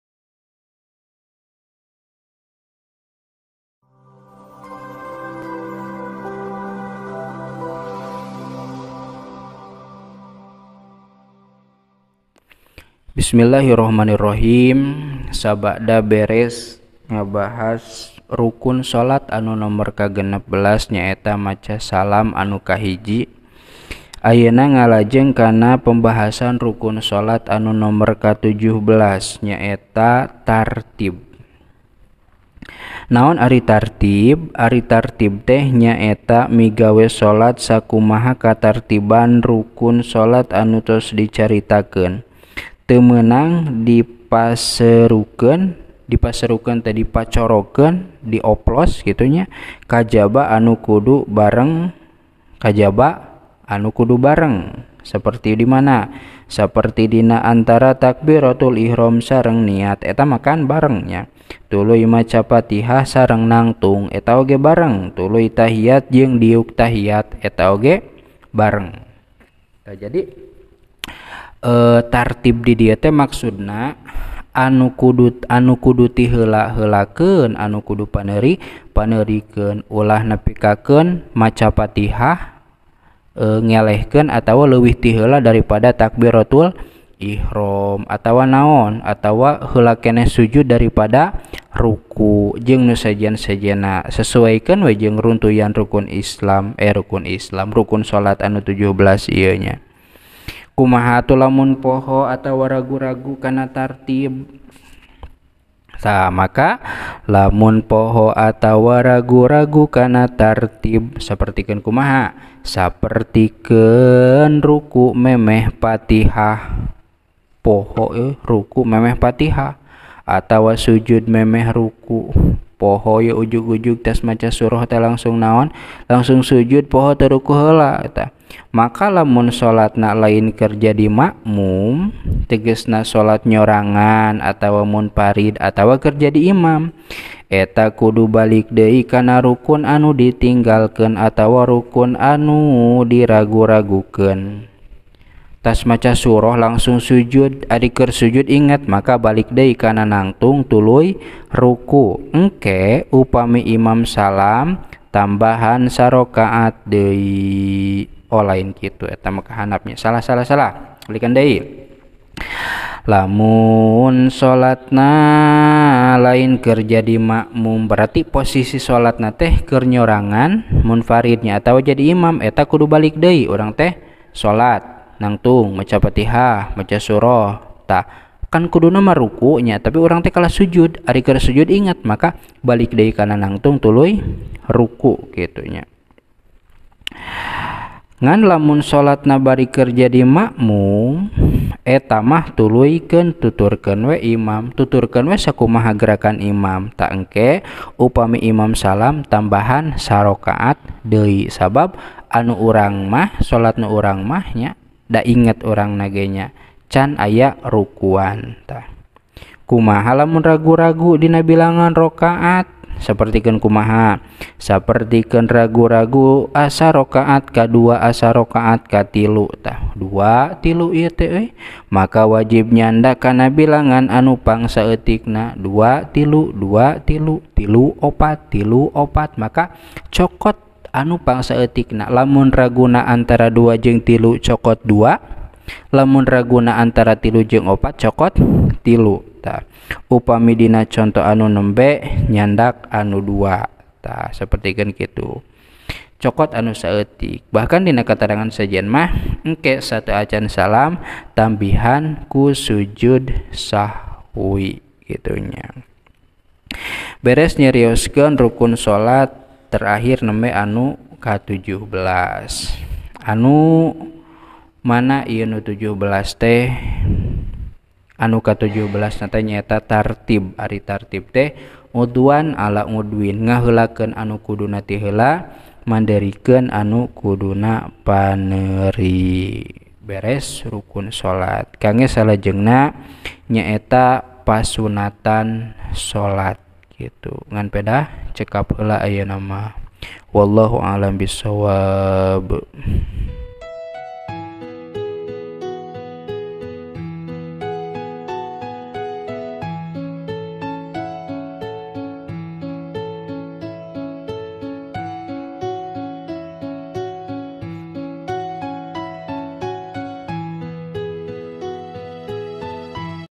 Kajian Fiqih Dasar Ep. 121 - Rukun Sholat | 17. Tartib
Assalamu'alaikum...

Dalam video ini berisi kajian fiqih dasar dalam bahasa sunda yang di ambil dari buku manqulah "FIQH KITAB KUNING"
karya ABU SALMA (Khodimul Ma'had Ponpes Miftahul Khoer Pasirbatang Manonjaya Tasikmalaya Jawa Barat)
yang referensi / mantuq nya di ambil dari kitab² fiqih seperti Fathul Mu'in, Bajuri, Al Maqoyis Wal Maqodir, I'anatutholibin, DLL.
Selain bertujuan untuk hafalan, juga jadi sebuah kenangan di masa yang akan datang.

Note :

(✓) Semoga bermanfa'at;
(✓) Di tunggu kritik & saran nya demi perbaikan di masa yang akan datang;
(✓) Terimakasih.

By.